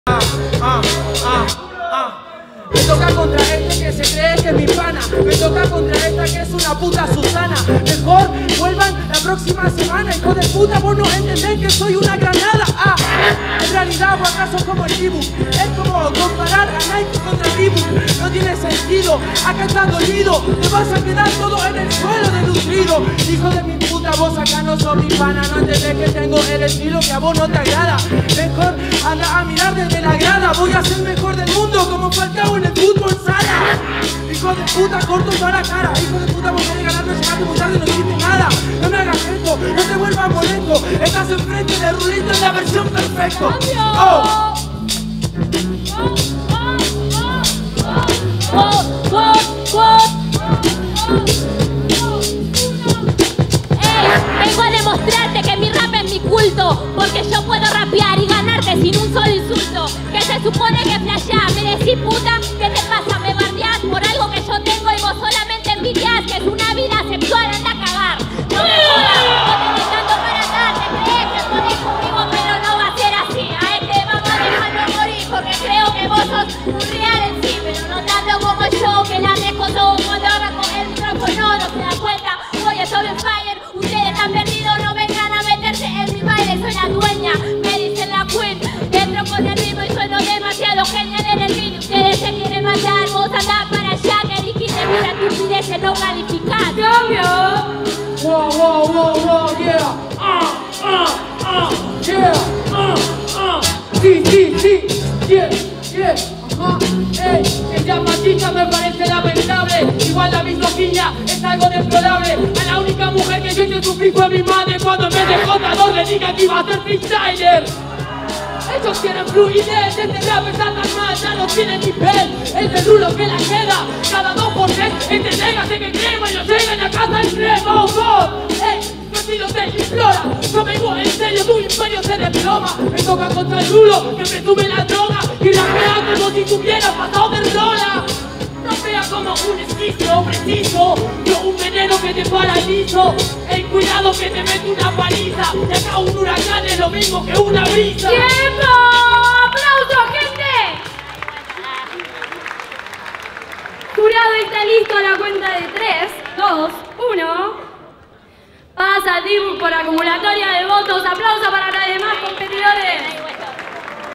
RULO vs ABBY vs DYBBUK - 8vos Fecha 2 Torneo verano 2019 CULTURA RAP
“Cultura Rap” es la competencia de verano de freestyle 1vs1 organizada en el Centro Cultural Recoleta por Juancín (de El Quinto Escalón), en la que nuevos raperos vienen a forjarse un nombre dentro de la escena y competir por un viaje a Santiago de Chile a la DEM (Dame Ese Micrófono), la compe de plazas más grande de Hispanoamérica.

Todos los domingos de enero y febrero hasta el 3 de marzo a las 17 en el Patio del Aljibe, Centro Cultural Recoleta, Junín 1930, Ciudad de Buenos Aires.